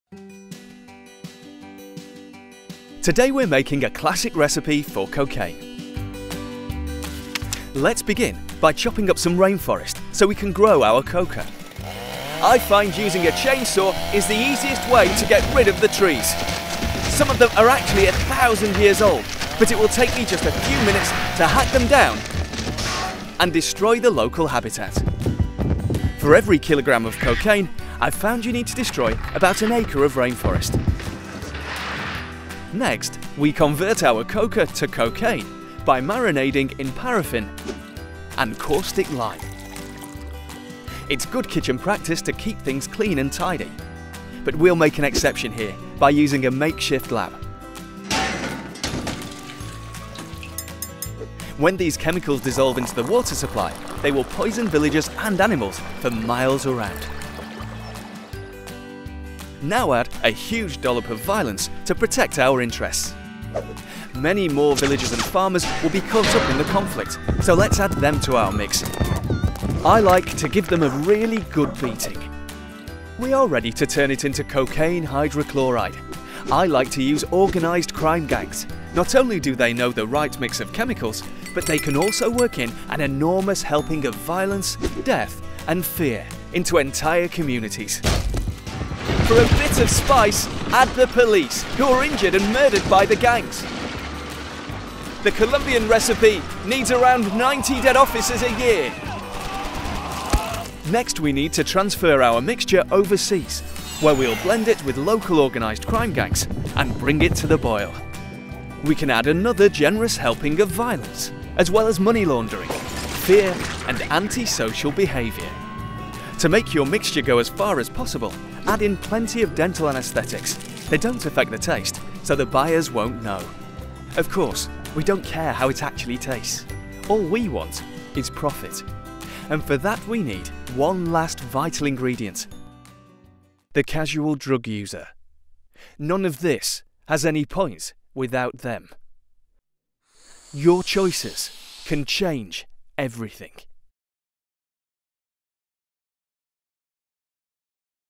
National Crime Agency - How to make cocaine - Social Media Campaign
Cocaine production is one of the most violent and environmentally destructive industries on earth. Many recreational users consider drug use to be a personal choice - as long as you're not hurting anyone then where's the harm? Well, EveryLineCounts. Every gram of cocaine you buy, every line you snort makes the most violent and destructive criminals on earth just a little bit richer.